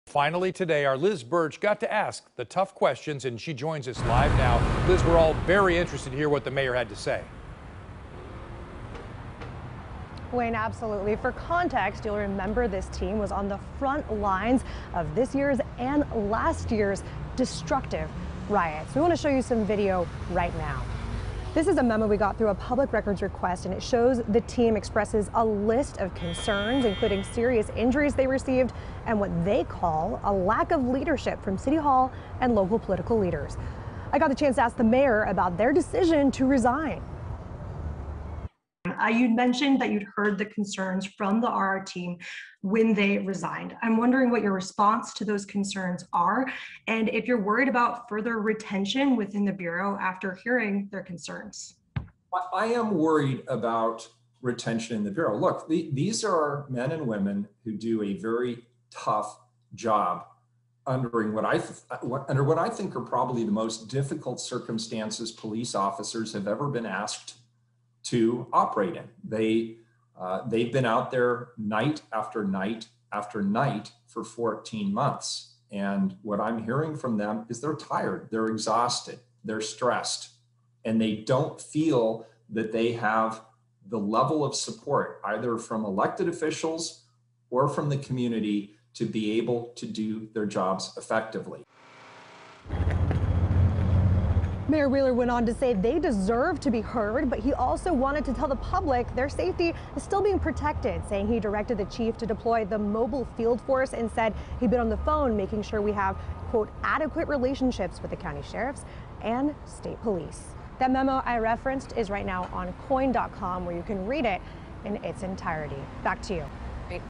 PPB changing procedures for traffic stops, consent searches
Portland Mayor Ted Wheeler and PPB Chief Chuck Lovell are expected to announce "two significant procedural changes" within the police bureau during a press conference Tuesday.